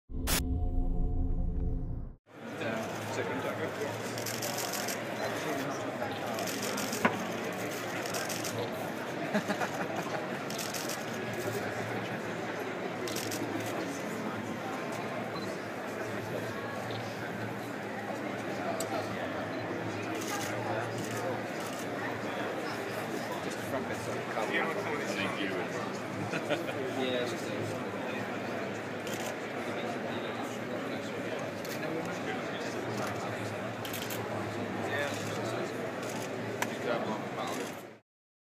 Mark Donnelly Pirelli Star Drive Citroen DS3 R3 Reveal
Mark Donnelly's Pirelli Star Drive Citroen DS3 R3 revealed at Autosport International, Birmingham. Mark will drive this car on the British Rally Championship in 2013.